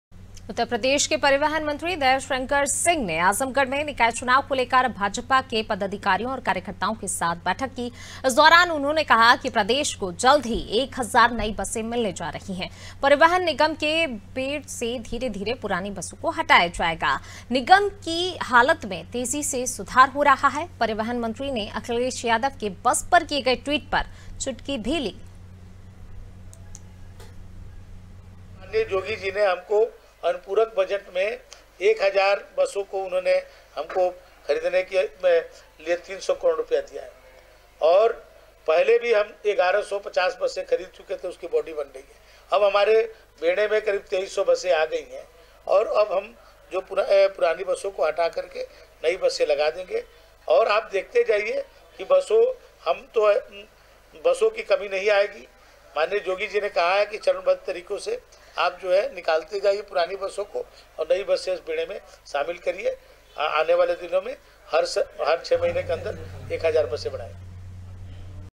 Azamgarh : परिवहन मंत्री दयाशंकर सिंह का दौरा, कार्यकर्ताओं से निकाय चुनाव पर की चर्चा
उत्तरप्रदेश के परिवहन मंत्री दयाशंकर सिंह ने आजमगढ़ में..निकाय चुनाव को लेकर भाजपा के पदाधिकारियों और कार्यकर्ताओं के साथ बैठक की...इस दौरान उन्होंने कहा...कि प्रदेश को जल्द ही एक हजार नई बसें मिलने जा रही है...परिवहन निगम के बेड़ से धीरे-धीरे पुरानी बसों को हटाया जाएगा...निगम की हालत में तेजी से सुधार हो रहा है...परिवहन मंत्री ने अखिलेश यादव के बस पर किए गए ट्वीट पर चुटकी भी ली...